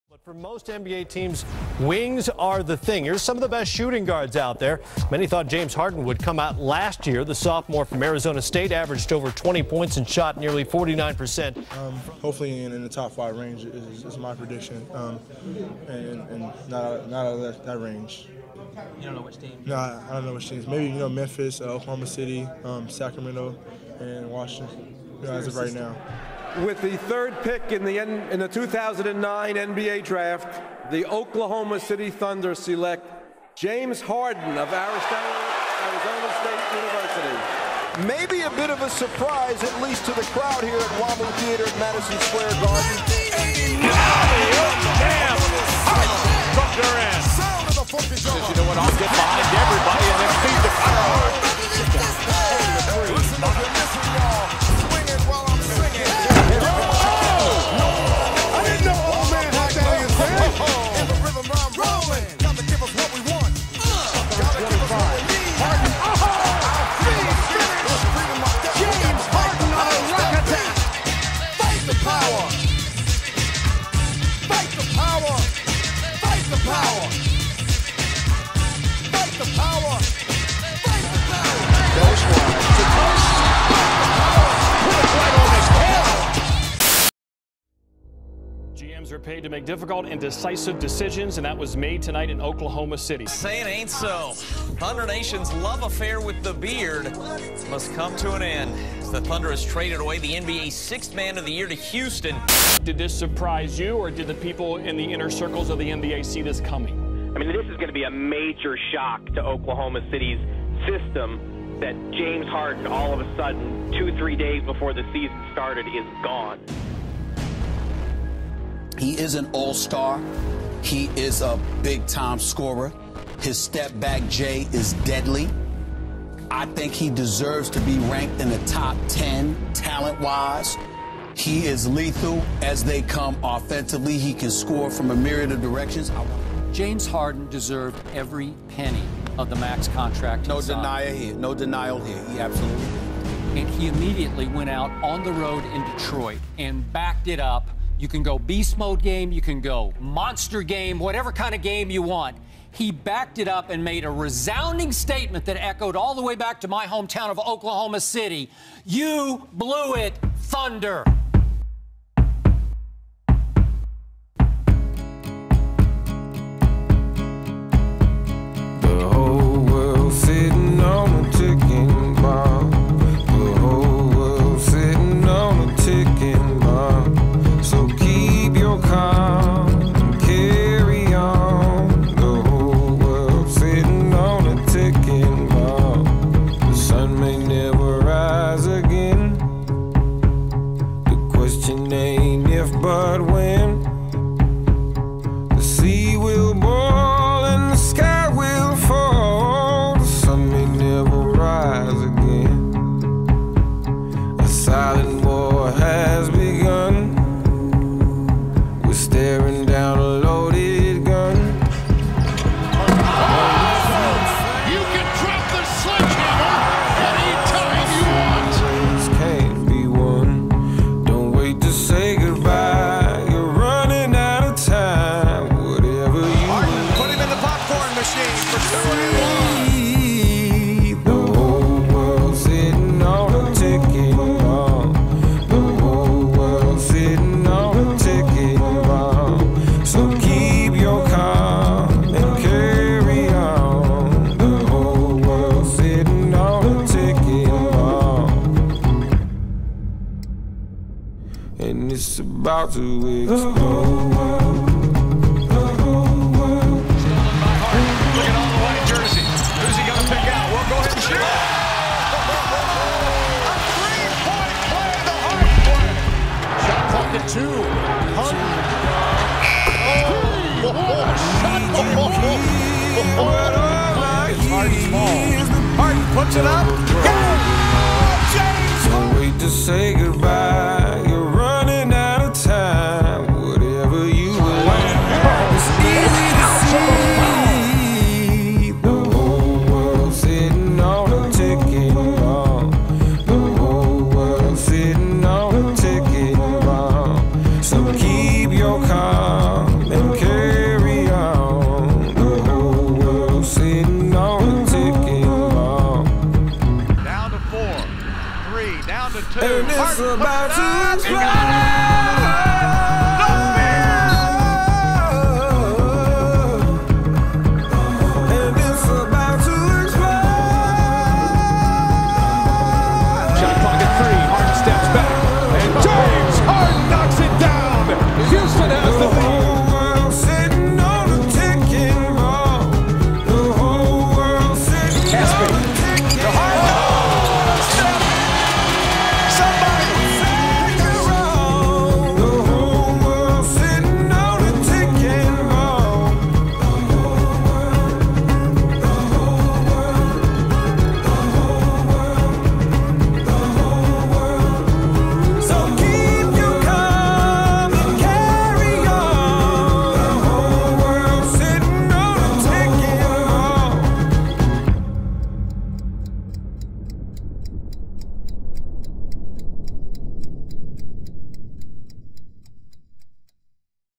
James Harden "Unguardable" HD
James Harden is without a doubt, one of, if not THE best player this season. Possible #1 candidate for league MVP. I made this mix to showcase his career thus far! :)